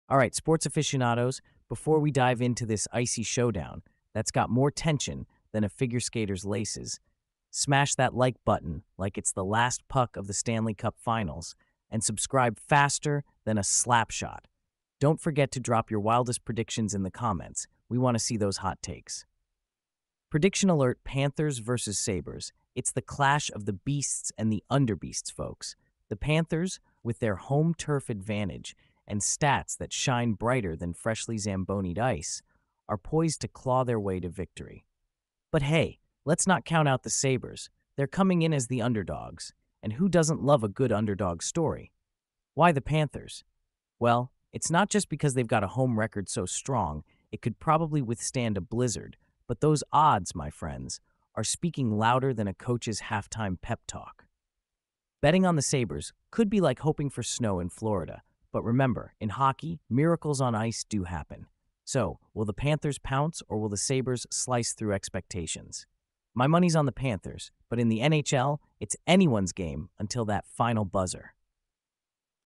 Buffalo Sabres VS Florida Panthers Prediction 2024-02-27
In the highly anticipated NHL showdown set for February 27, 2024, the Florida Panthers are poised to host the Buffalo Sabres in a clash that's generating buzz among ice hockey aficionados. With the Panthers favored by bookmakers and the Sabres coming in as underdogs, the stage is set for an electrifying encounter at the FLA Live Arena.

The Panthers, with moneyline odds of -225, are the clear favorites to win this matchup, while the Sabres trail with odds of +185. The spread sees the Sabres at +1.5 and the Panthers at -1.5, with an over/under set at 6.5 goals, indicating expectations of a moderately high-scoring game.

Weather in Buffalo on game day is forecasted to be considerably cloudy with occasional rain showers in the afternoon, potentially impacting the Sabres' travel and pre-game preparations. However, the real storm will be inside the arena as these two teams collide.

The Panthers boast a formidable record of 38-16, with a particularly strong performance at home, making them the Goliaths of this encounter. The Sabres, on the other hand, hold a 27-27 record, embodying the role of David with a chance to cause an upset. In NHL history, underdogs have a win percentage that can't be ignored, suggesting that while the odds favor the Panthers, the Sabres could still pull off a surprise.

Given the Panthers' impressive home record, the strength of their lineup, and the backing of the bookmakers, my prediction leans heavily towards a Panthers victory. However, in the spirit of hockey, where the puck can bounce in unexpected ways, the Sabres are not to be underestimated. Expect a hard-fought game, but the Panthers' prowess should ultimately prevail, making them the pick of the day.

So, grab your popcorn (or, given the weather, maybe a hot cocoa) and brace yourself for an NHL night that promises to deliver thrills, spills, and possibly a few chills. May the best team win, but all bets are on the Panthers roaring to victory in their own den.